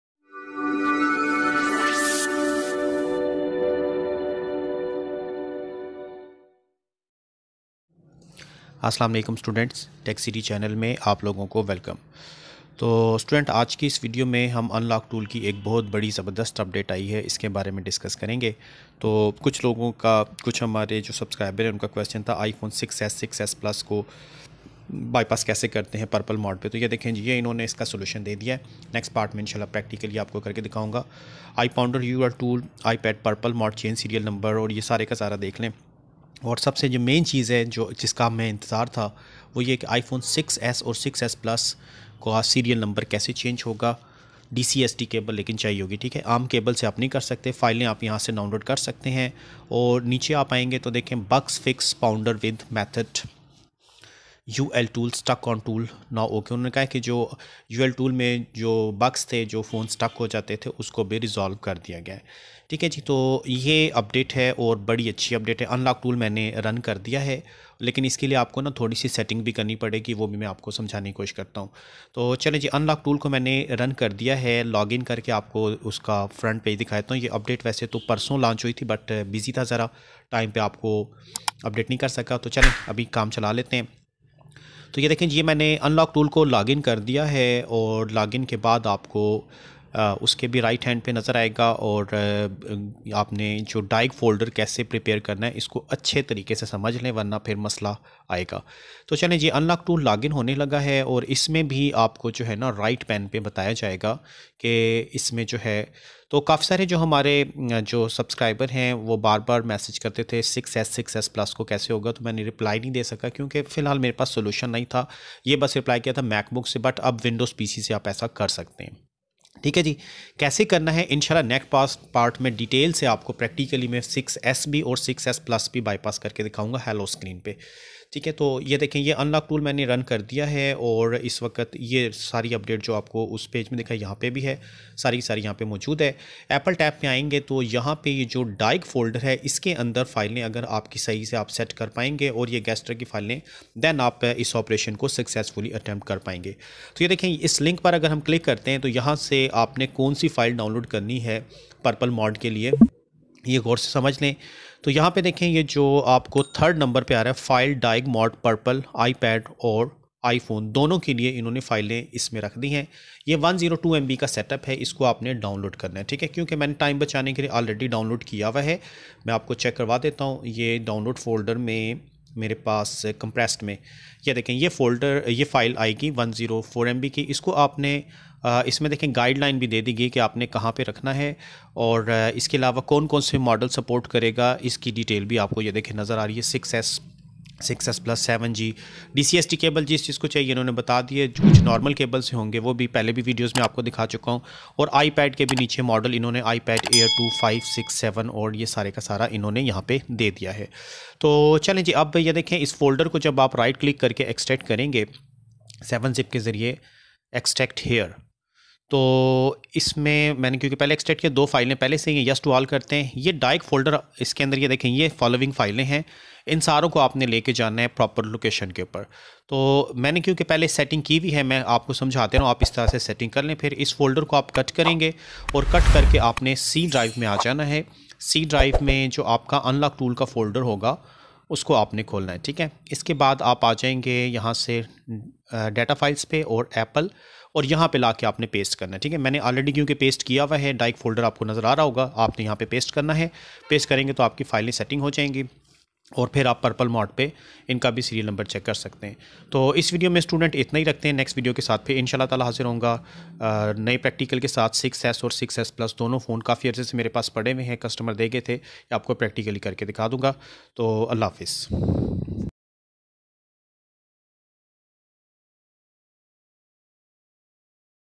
Unlock Tool New Big Update 2023 add Ipad pro, 5th,6th,7th gen iphone 6s, 6SP bypass PWNDFU updated
This video we discuss the new big update of unlock tool which added the following new features:

UnlockTool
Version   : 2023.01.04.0
Hash    : 89b57a8557718fa3dc3b3e55bdcf3936
Update on : Jan. 4, 2023, 4:33 p.m.
Change Log :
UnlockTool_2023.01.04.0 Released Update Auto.

Apple
- Added More New PWNDER ULTool
- iPad Purple Mode Change Serial
- iPad Air 2 Need Cable DCSD Change S/N
- iPad Pro (9.7-inch) Need Cable DCSD Change S/N
- iPad (5th Generation) Need Cable DCSD Change S/N
- iPad (6th Generation) Only Cable Lightning Change S/N
- iPad (7th Generation) Only Cable Lightning Change S/N
- iPhone 7G/7P Now Supported Diag PWNDFU with ULTool
- iPhone 6S/6SP Need Cable DCSD Change S/N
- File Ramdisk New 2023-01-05
Bugfixes
- Bugfixes PWNDER with method ULTool stuck on Tool now ok